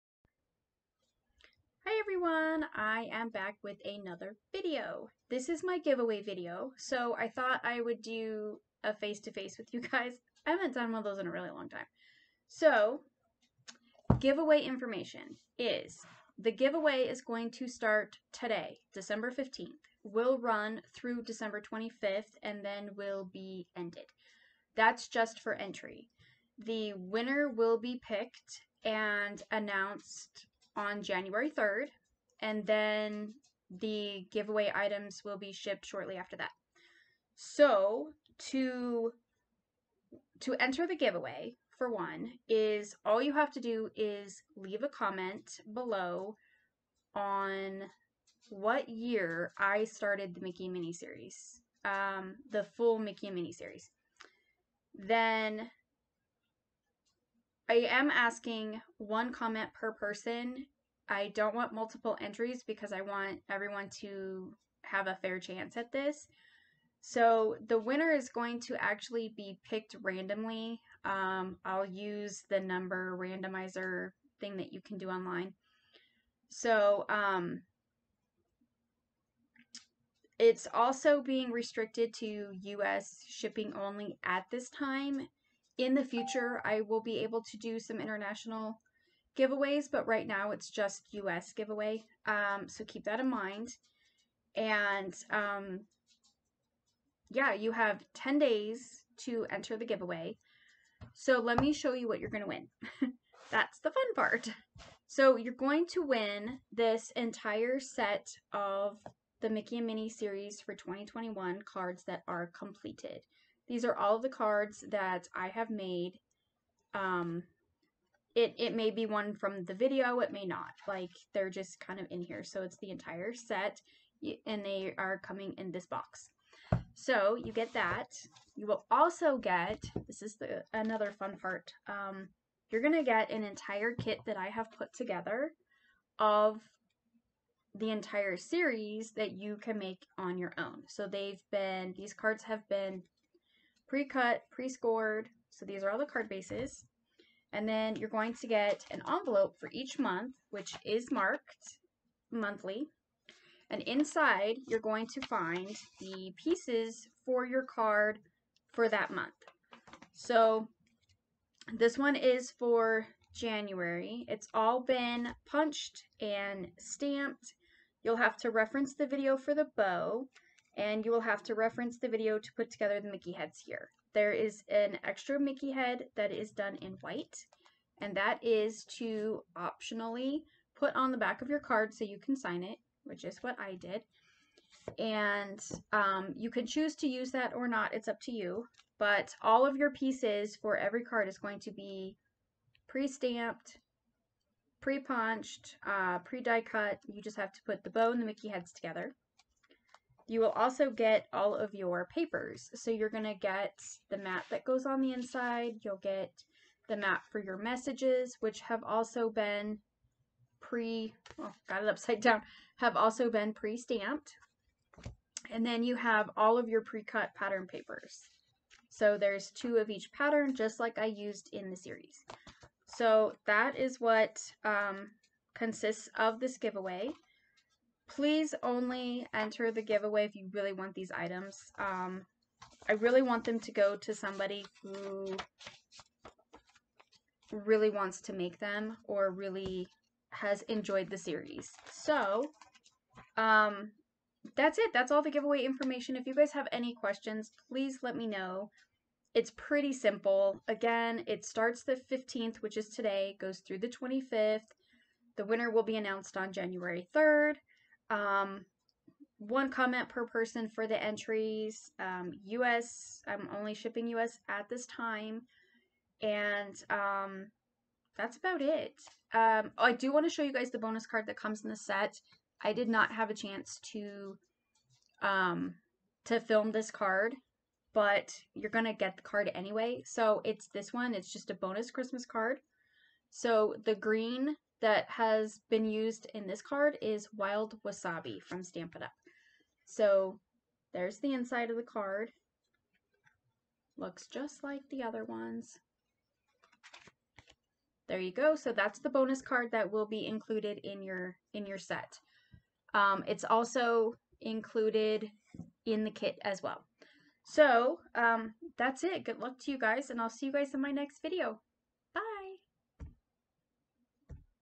Mickey and Minnie Card Series 2021 - Giveaway CLOSED
!!ღღღღღ Expand for more info ღღღღღ!!

Will only run for 10 days. December 15th - 25th 2021
One comment per person for entry
Winner will be chosen and announced on January 3rd 2022
Shipping to US only at this time
Good Luck! :)